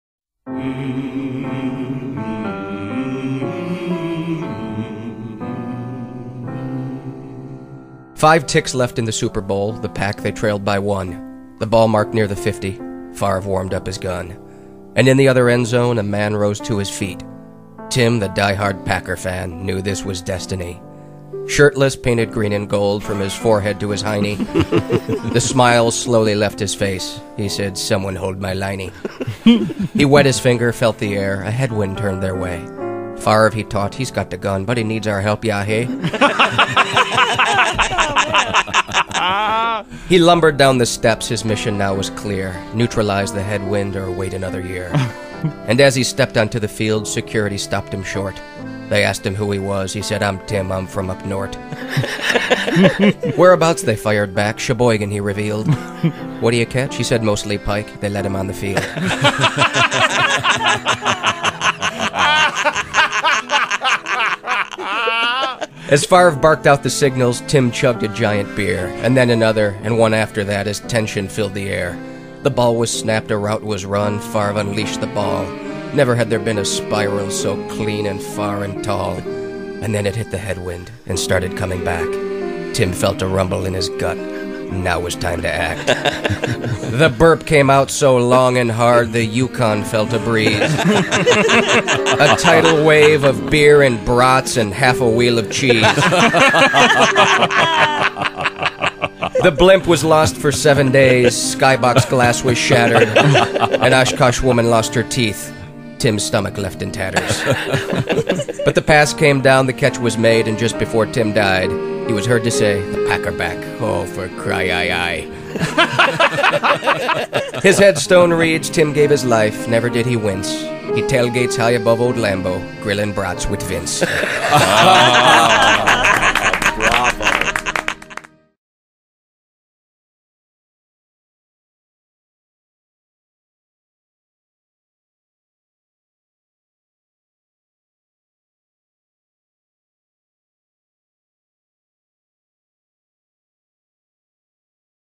Tim the Diehard Packer Fan
A great little poem about a packer fan who goes out of his way to help favre win the big one!